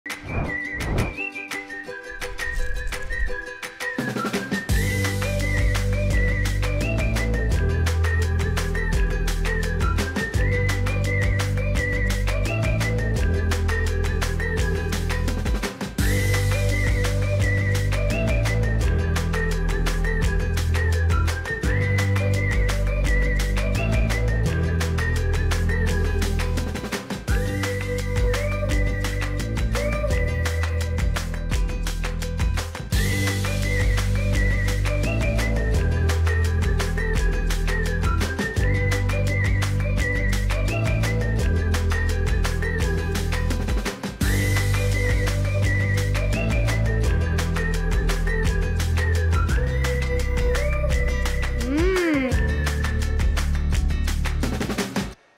Flatbread Provençal | Recipe | EN
Have you ever thought about cooking your own bread ?🍞
The Provencal Flatbread is so fun to make and will make your last summer days really tasty😜. Topped with fresh herbed cheese🧀, black olives🫒, tomatoes🍅 and a pistou sauce, this dish is perfect to be eaten on its own or to accompagny a meal or an appéritif.🥂